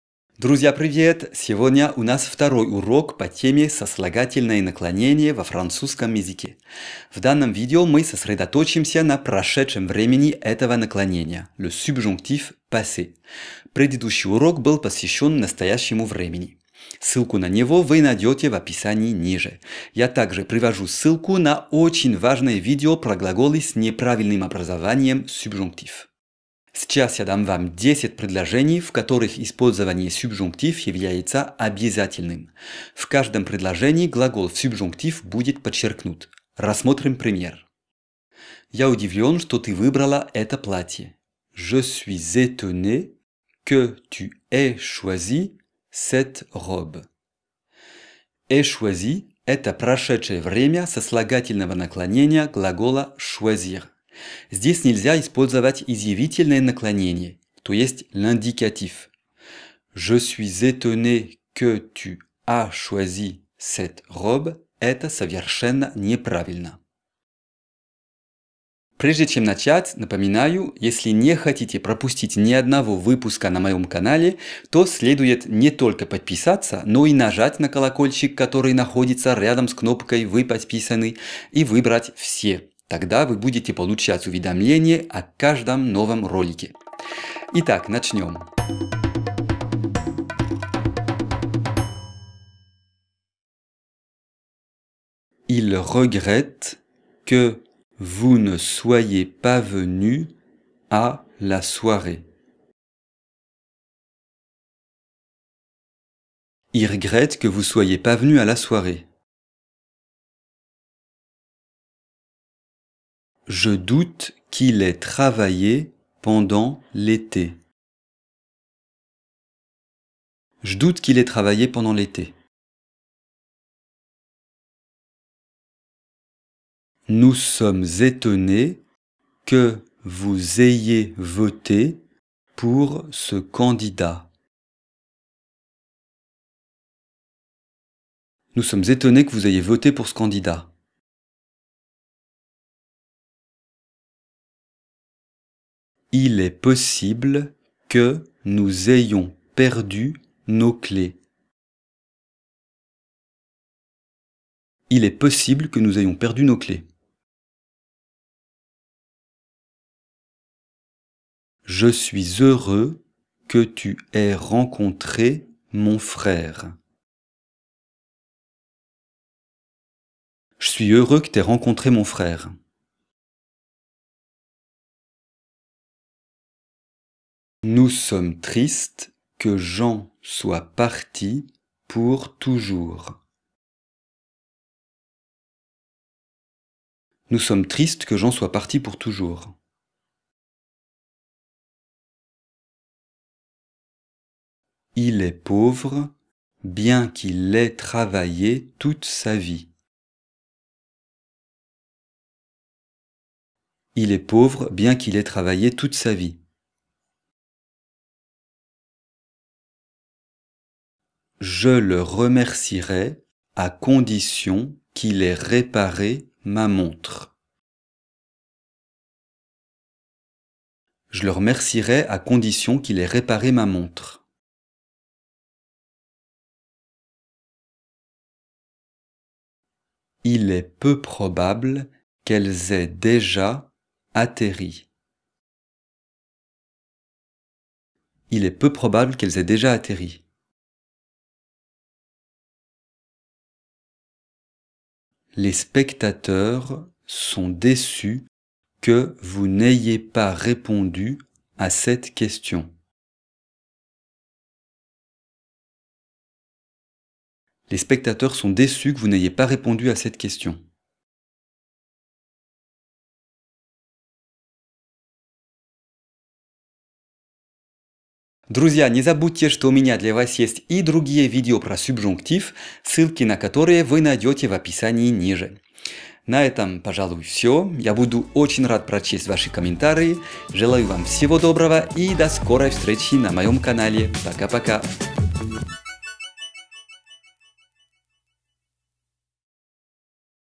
Сослагательное наклонение (subjonctif) во французском языке | Прошедшее время
Прошедшее время cослагательного наклонения (le subjonctif passé) во французском c носителем языка. В этом видео, я даю вам 10 предложений с глаголом в subjonctif (сюбжонктив) passé.